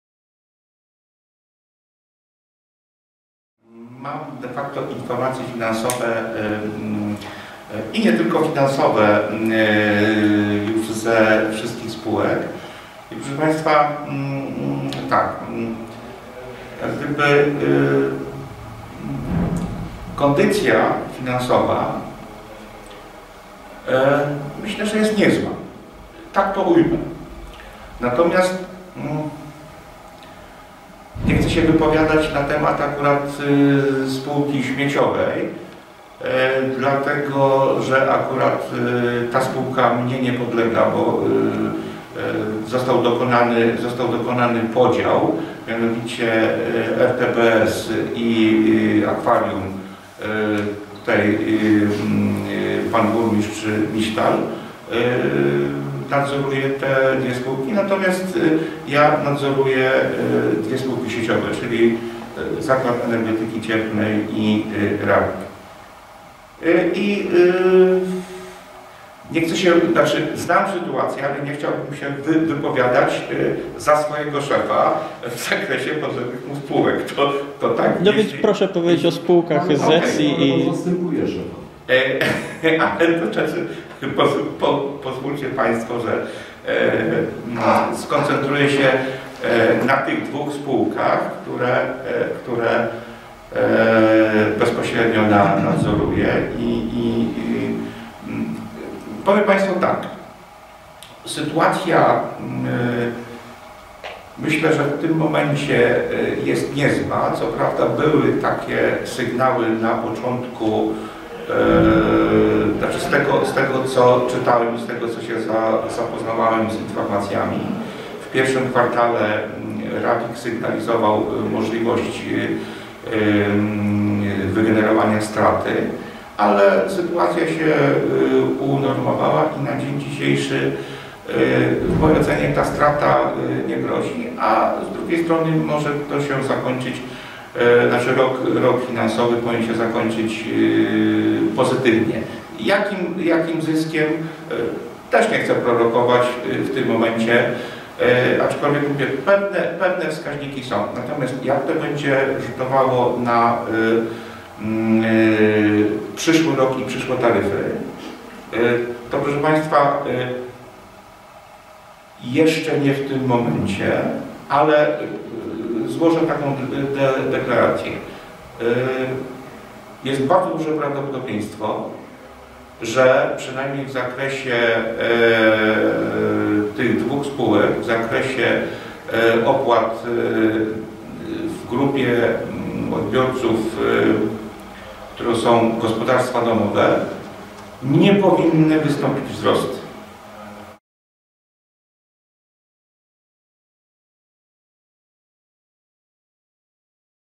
Cena wody i ścieków w Rawie Mazowieckiej 2 /
Janusz Marek Pastusiak, wiceburmistrz Rawy Mazowieckiej o cenie wody i ścieków w Rawie Mazowieckiej na 2016 rok. Wypowiedź z 22 września 2015 roku.